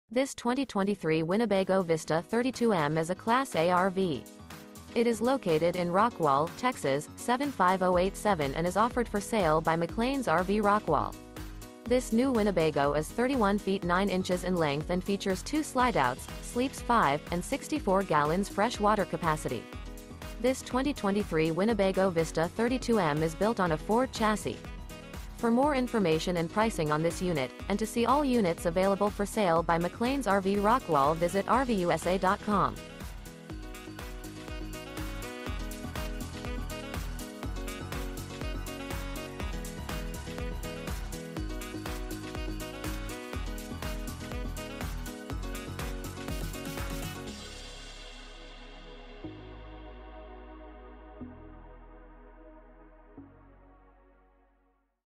Beautiful 2023 Winnebago Vista 32M Class A RV For Sale in Rockwall, TX | RVUSA.com
This 2023 Winnebago Vista 32M is a Class A RV. It is located in Rockwall, TX 75087 and is offered for sale by McClain's RV Rockwall. This New Winnebago Is 31' 9" in length and features 2 slideouts, sleeps 5, and 64 gal fresh water capacity. This 2023 Winnebago Vista 32M is built on a Ford chassis.